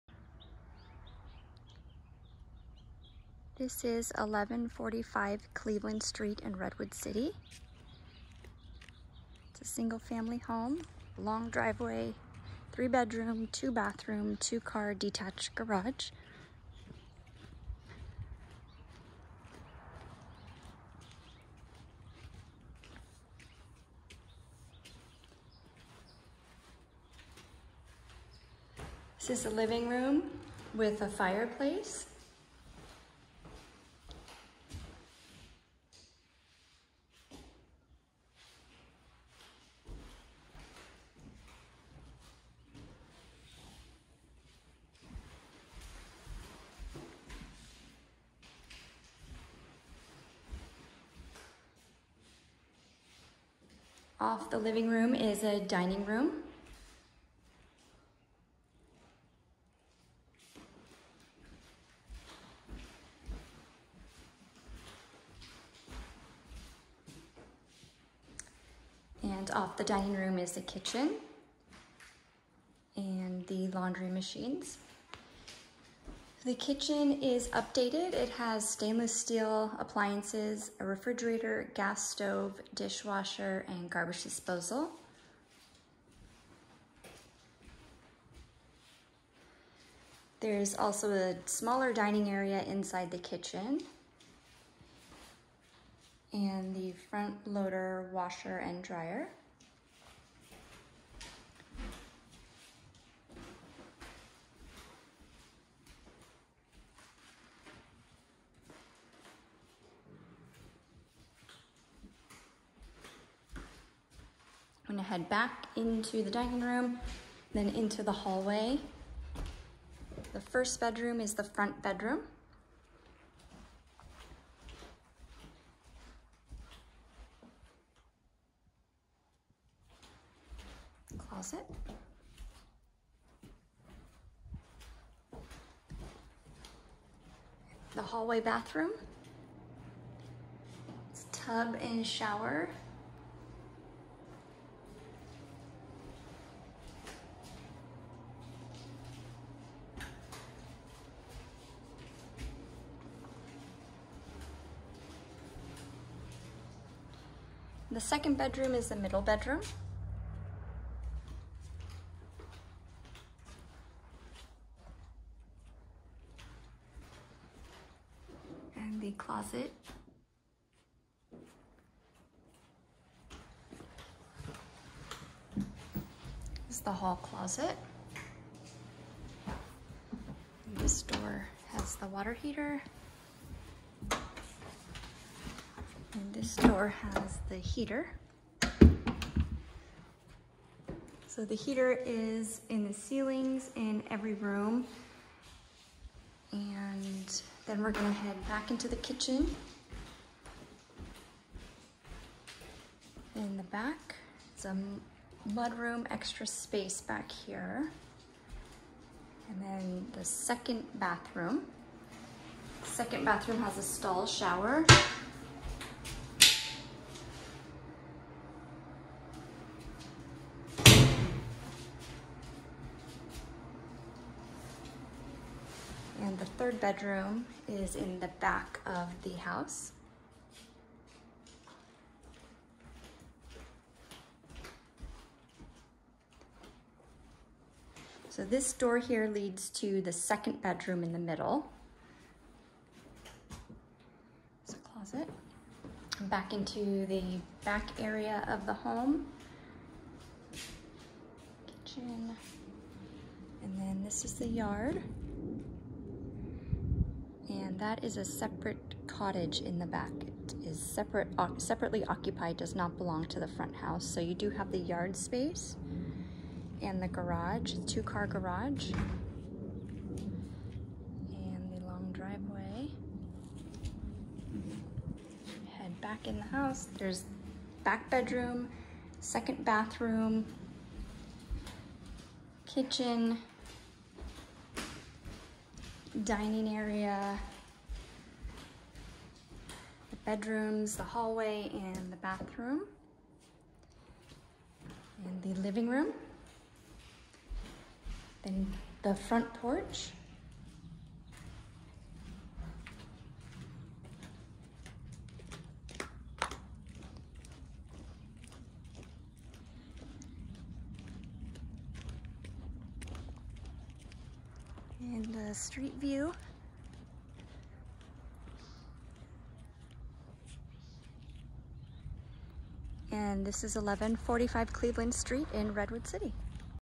Rental Available: 1145 Cleveland St Redwood City CA 94061
Cross Street: Roosevelt Ave

***Due to COVID-19 we will be conducting virtual showings first / YouTube video then scheduling appointments to view the property in person. All in person showings must adhere to local and state health guidelines.***

The FACTS
* 3 bedrooms
* 2 bathrooms
* 2 car garage
* Living space: 1450 square feet +/-
* Note: This is a single family home with a cottage in the back. The cottage is separate an d occupied.

The DETAILS
Spacious home with charming details and glamorous updated kitchen! Quaint porch perfect for a small sitting area. Enter into living room with large window that allows natural light in, and fireplace. Formal dining room off living room and kitchen. Gorgeous kitchen with stainless steel appliances-gas stove/oven range, dishwasher, refrigerator, beautiful counters and backsplash plus eat-in area or nook for desk.  Washer and dyer with folding table on top of it or additional counter top perfect for entertaining. Three bedrooms with carpet. Hallway bathroom with shower over tub. Second bathroom with stall shower. Mud room in back of home, for extra storage space. Refinished beautiful dark hardwood floors in living room, dining room and kitchen. Long driveway with two car garage. Yard space in back perfect for a table and a small garden. Note: This is a single family home with a cottage in the back. The cottage is separate and occupied.

The COMMUNITY:
* CalTrain: 1.0 miles
* Downtown Redwood City (banks, Philz Coffee, movie theater, restaurants): 1.0 miles
* Box: 1.0 miles
* Sequoia Station (grocery shopping, Starbucks, restaurants, store): 1.0 miles
* San Francisco Airport: 16.2 miles
* Google: 10.4 miles
* Stanford: 5.5 miles
* Kaiser: 1.6 miles
* EverNote: 1.5 miles
* Red Morton Park: 0.7 miles

The TERMS
* Rent: $3575
* Deposit: $3600
* Tenant pays all utilities
* Gardener included
* 1 year lease
* No pets
* No smoking
* No AirBnB/No subletting

Interested or Questions:
Denette Bartoli
Terrace Associates
DRE License #01759625
Follow Property Shop Silicon Valley on Instagram!